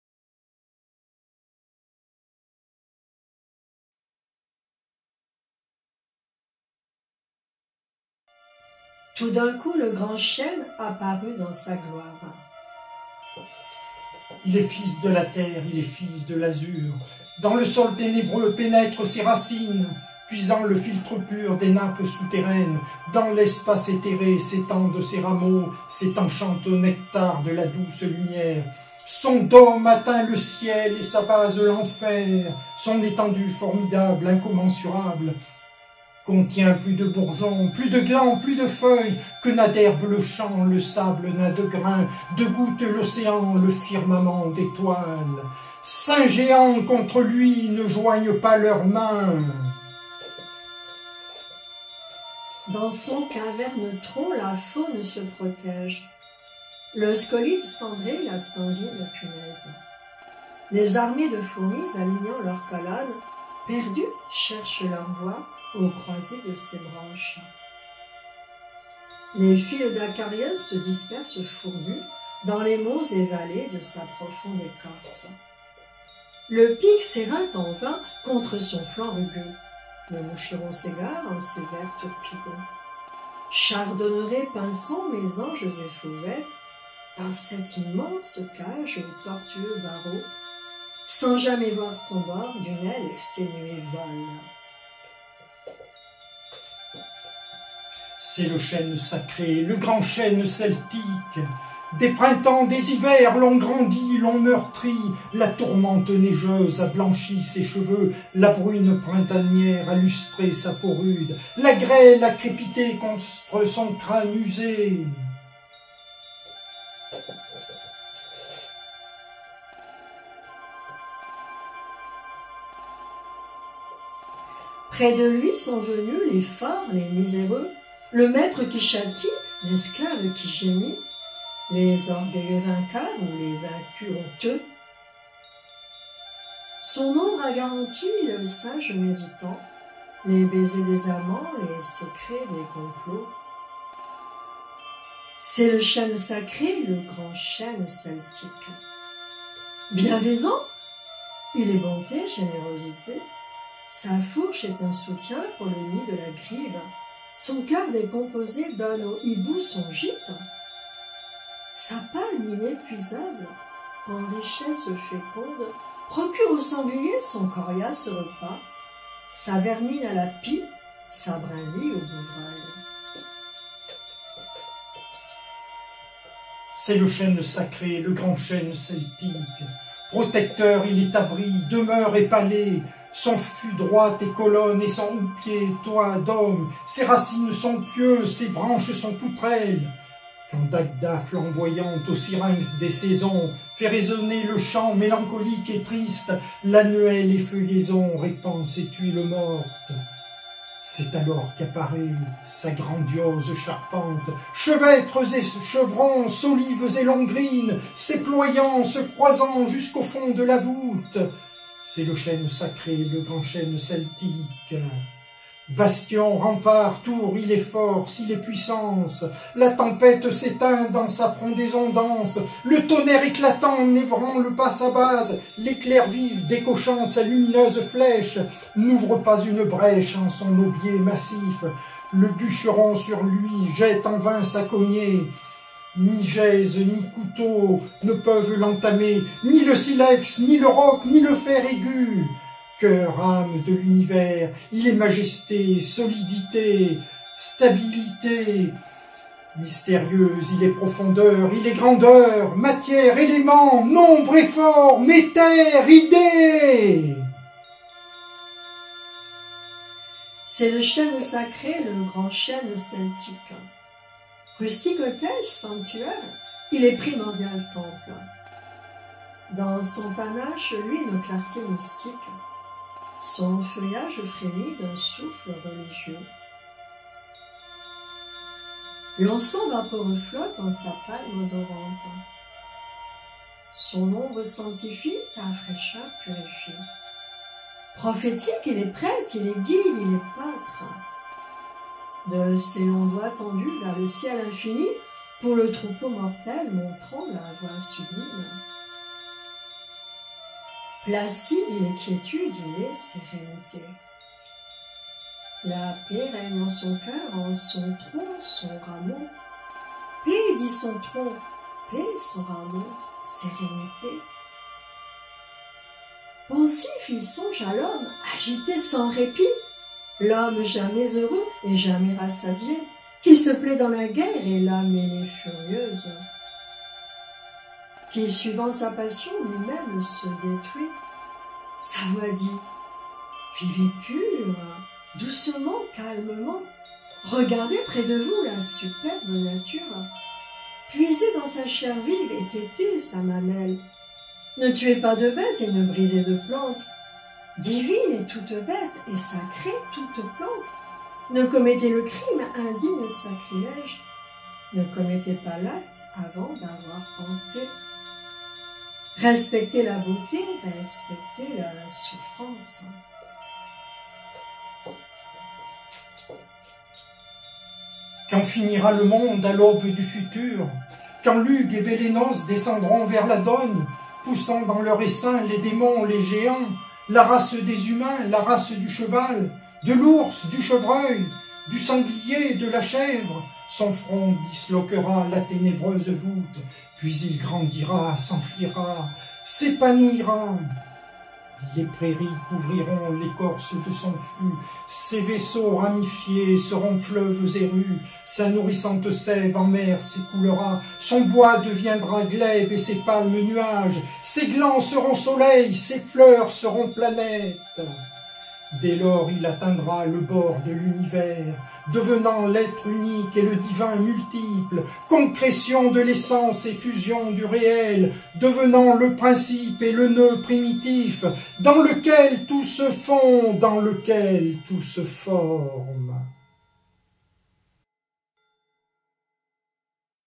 Le grand Chêne celtique - Poème épique
Extrait de "Saga celtique" (Niew, prêtresse celte)
Poème épique évoquant la grandeur du chêne celtique selon sa dimension concrète et mystique...
Déclamation: Josyane Moral-Robin, Claude Ferrandeix